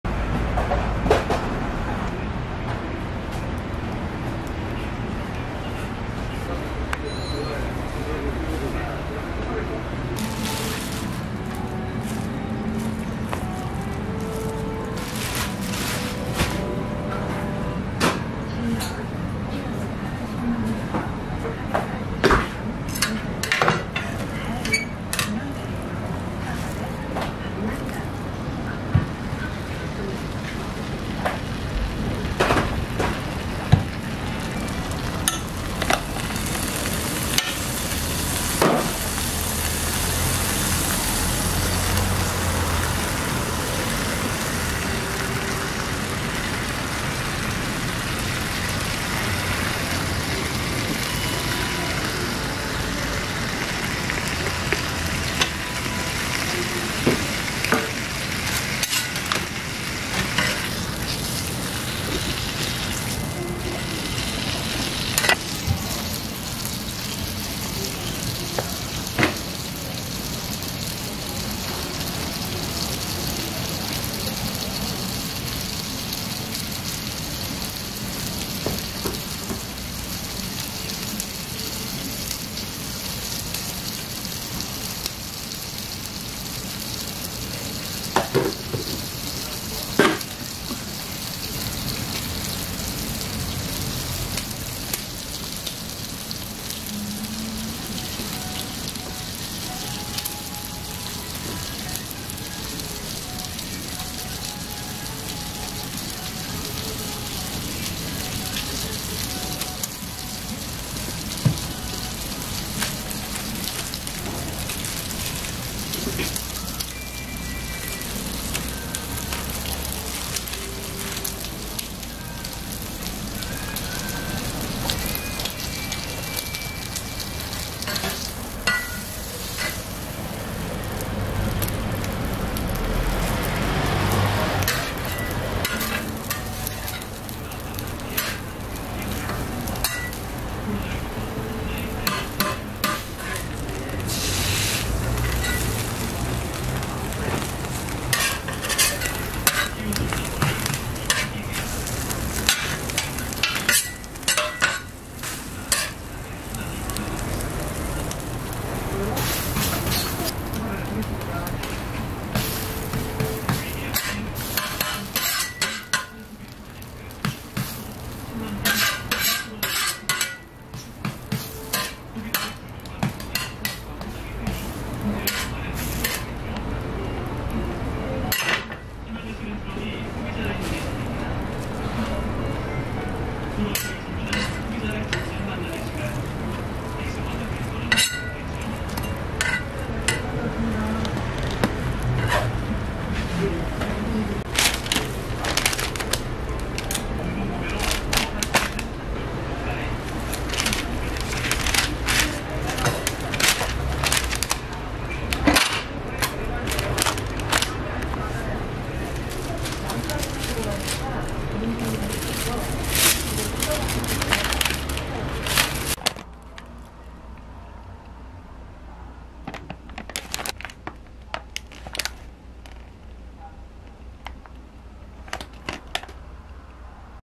How to make korean best toast, 오산에서 먹는 대한민국 대표 이삭토스트, 불고기, 베이컨 토스트, Korean Street Food
🔔 Subscription
💰 Price
KRW 3,000~4,000 won